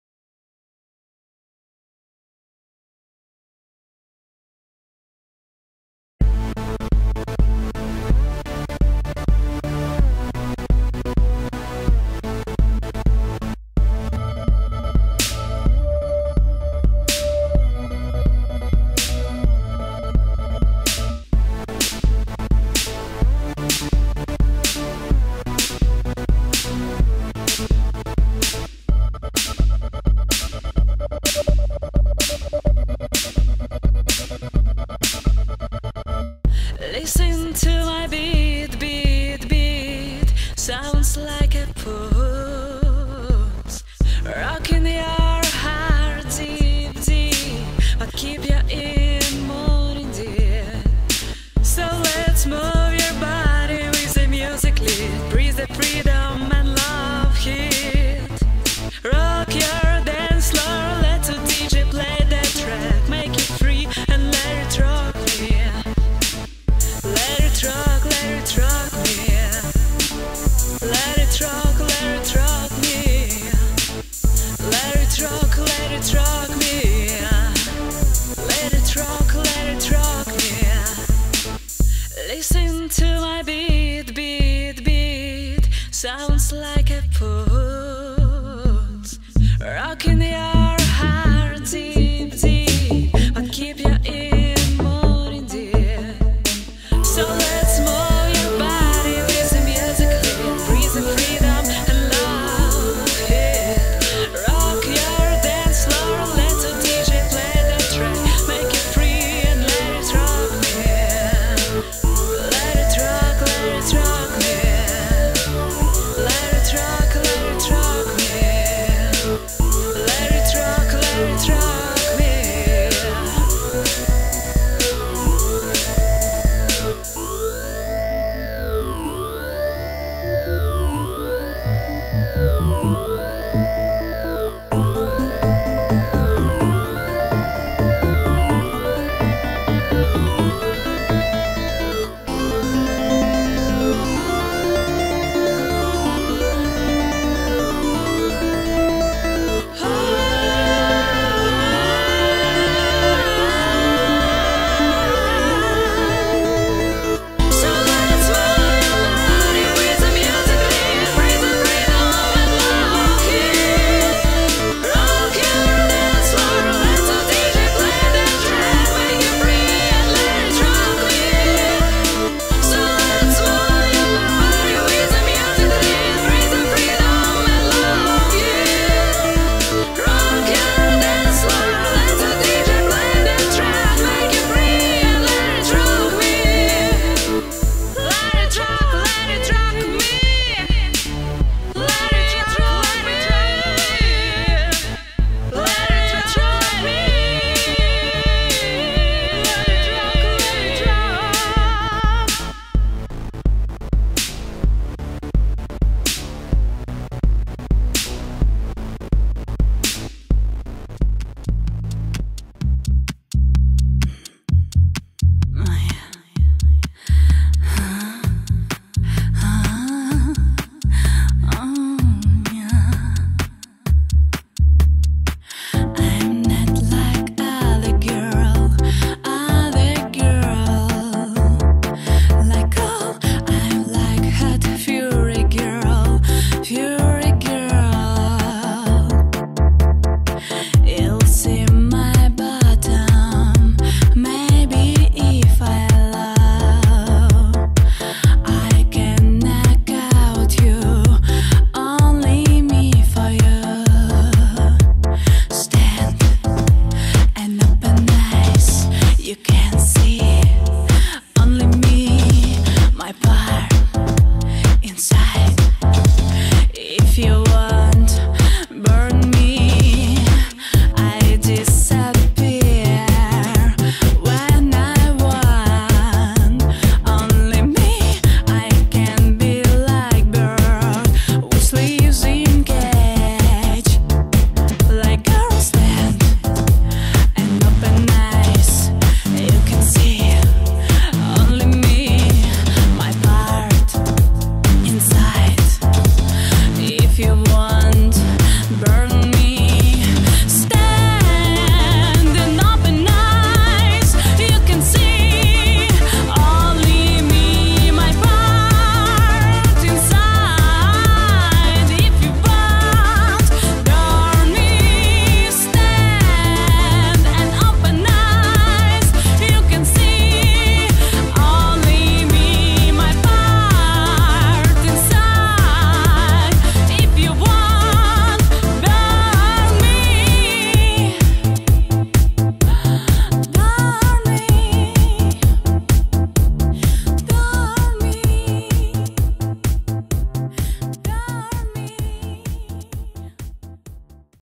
FIRST EUROPEAN FPV MULTIROTOR RACE IN AUSTRIA SPITZERBERG
DVR Aufnahmen
FIRST EUROPEAN FPV MULTIROTOR RACE IN AUSTRIA SPITZERBERG / EUROPA PREMIERE AERIAL GP FPV MULTIROTOR RACING EVENT
Musik by Kate Orange/Jamendo